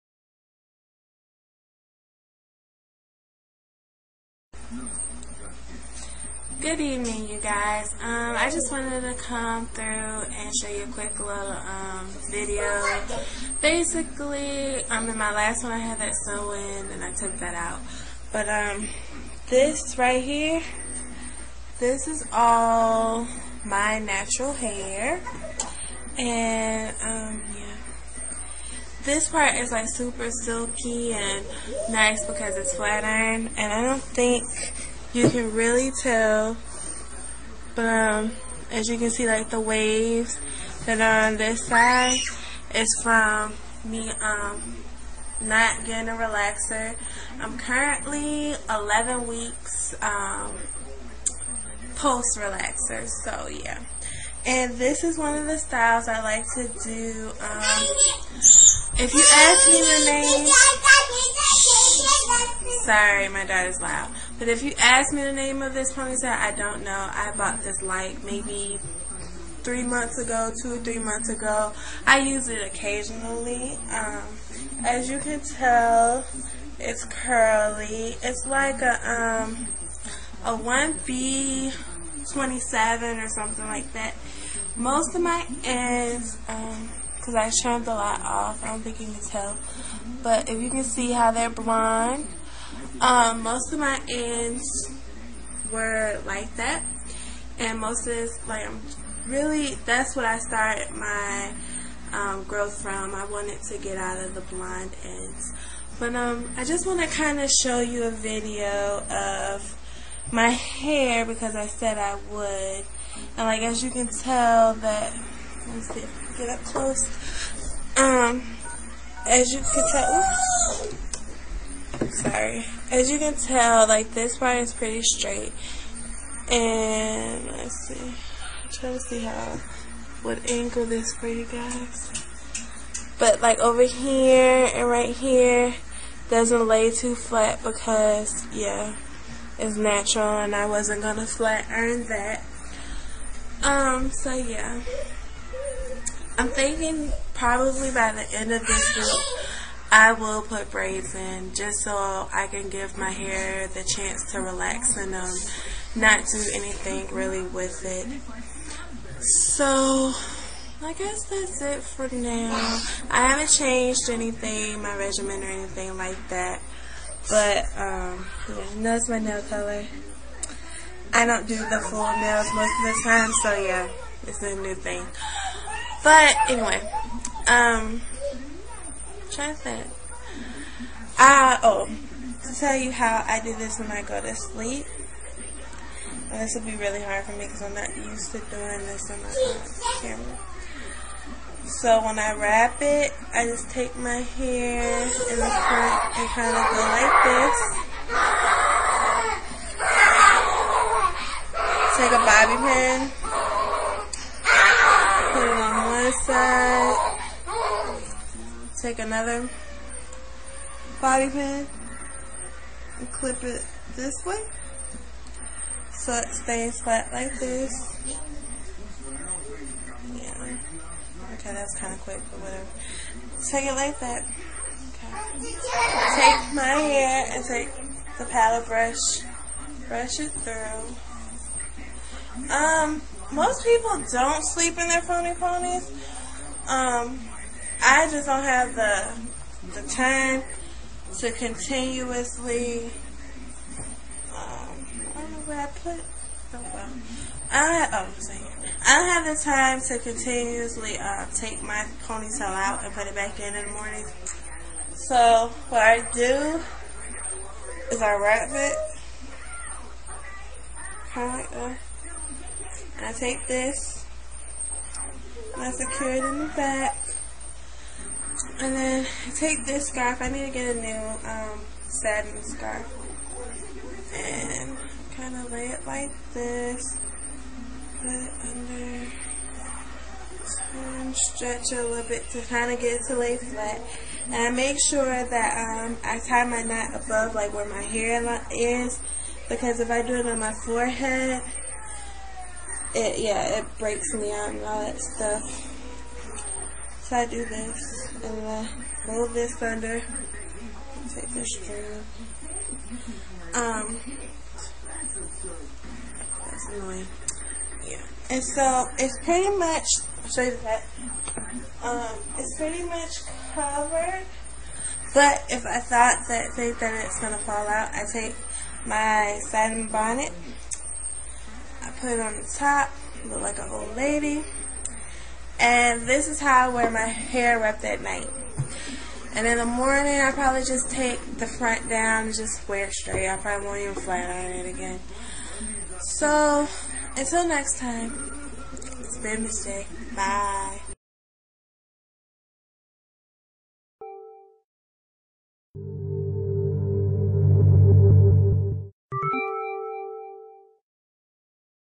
My Drawstring Pony
Just a quick look at my fake pony tail and to show you guys a view of my natural hair.

Updated: I found the name of the ponytail and I have included a link to buying it online.. The name of the ponytail is Lo Me Moon R/V. Its completely synthetic and it cost about $13. The color is F1B/30.